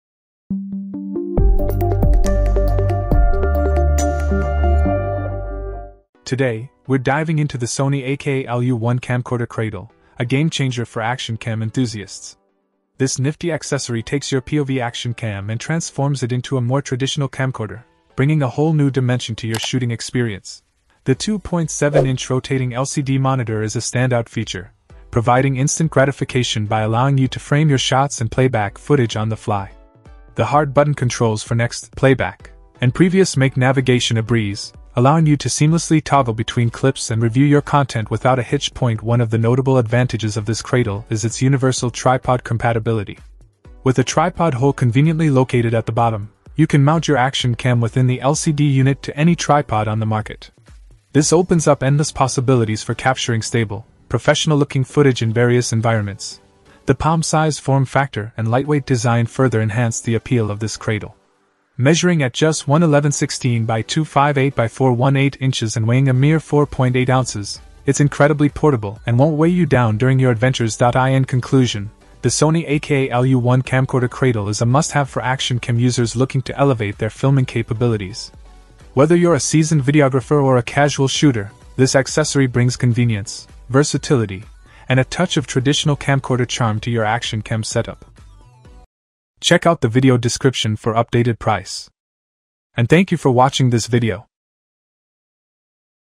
Transform Your Action Cam with Sony AKA-LU1 Camcorder Cradle: A Comprehensive Review!
0:00 Intro
0:06 Review

Things that we mentioned in this video:
Sony AKA-LU1 Camcorder Cradle : B00ARI98X0